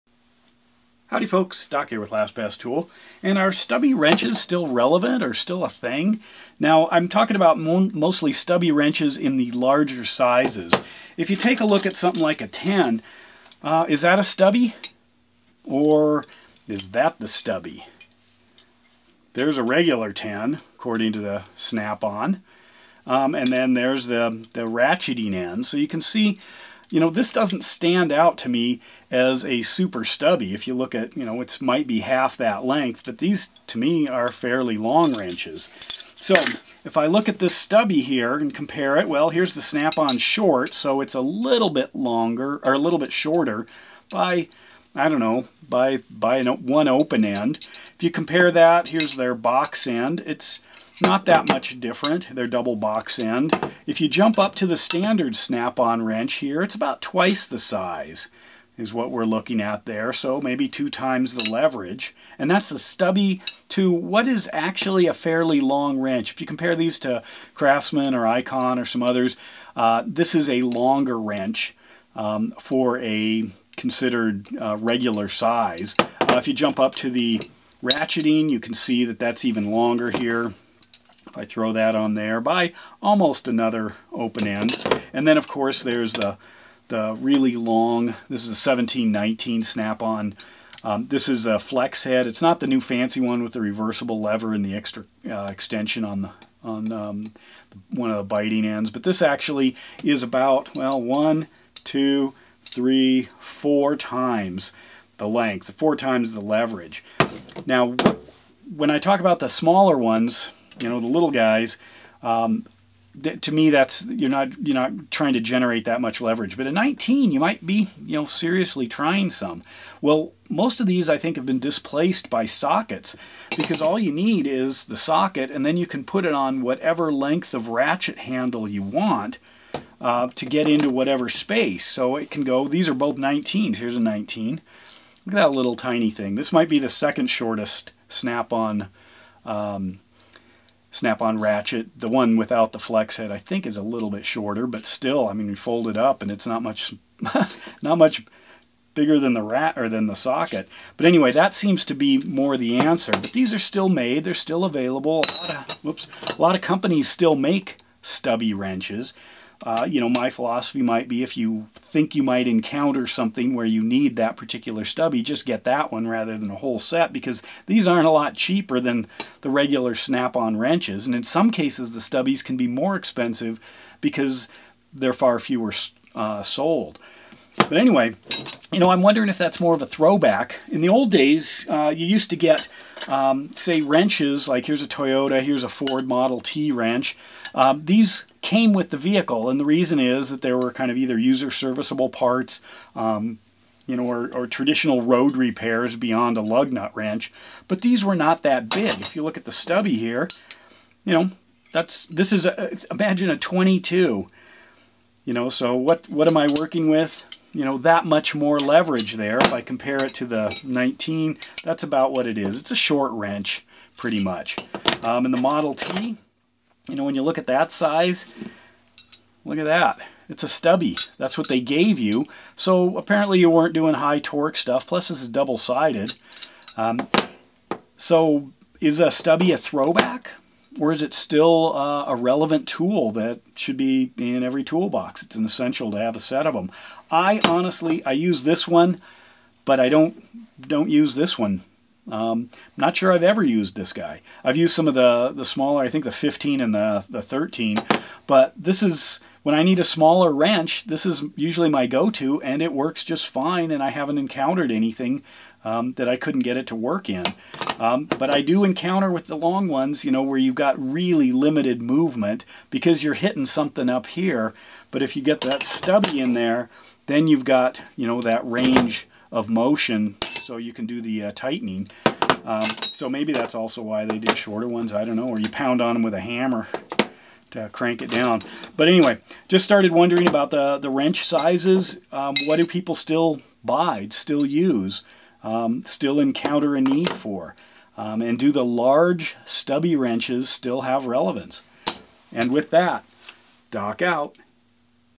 Are Stubby Wrenches Still Relevant? Especially at Snap On prices! Have ratchets killed off stubbies?
Are Stubby Wrenches Still Relevant? Especially at Snap On prices! Have ratchets killed off stubbies?Ratchets are the answer to almost everything especially stubbies.